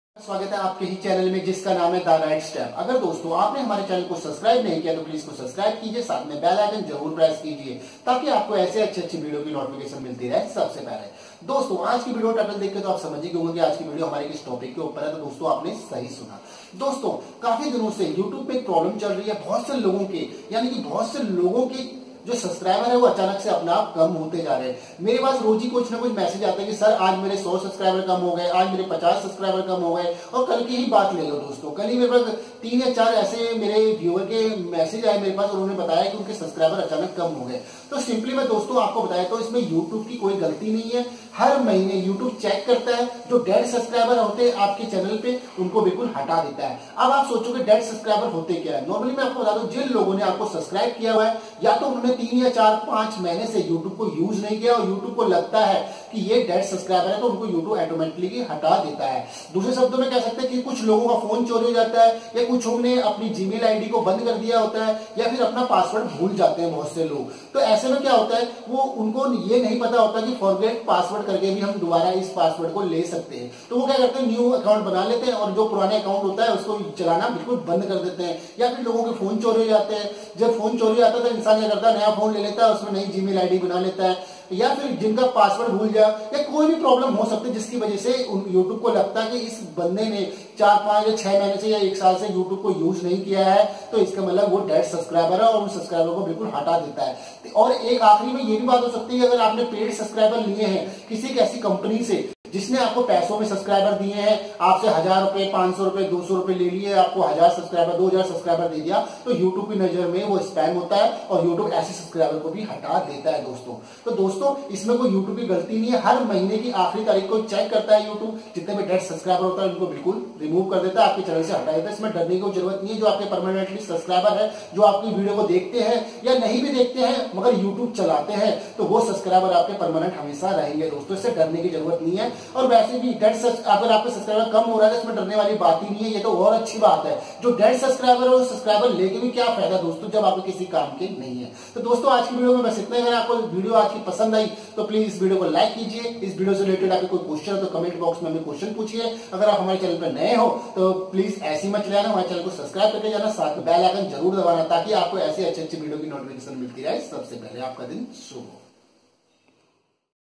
Why YouTube Subscribers Decrease | Why Decrease Your YouTube Channel Subscribers
नमस्कार दोस्तों
आपका स्वागत है आपके ही चैनल में जिसका नाम है द राइट स्टेप दोस्तों आज की वीडियो में हम आपको  ऐसी चीजें बताएंगे ज़िससे कि आप को बहोत फायदा हो

Aaj ki video main hum aap ko batyagai ki Ager aap ak Youtuber ho to ya video aap k lea bhout important hai.kuki ya jo problem es video main aap ko batanai jaa rahai h..aaj kal aap bhe face kar rahai hogai..kuki kuch dino se youtube par jitnai bhe youtuber h un k subscriber apnai aap kam hotai jaa rahai hai..aur vo subscriber apnai aap kam kyu ho rahai hai..es video main hum ko full explained kar k batygai..to video ko end tak jarur dekhai aur jaada se jaada logo ko share bhe karai taaki ya useful information sabko mil sakai...

My Tripod
Green Screen Backdrop
My Mic

Samsung M20 3+64GB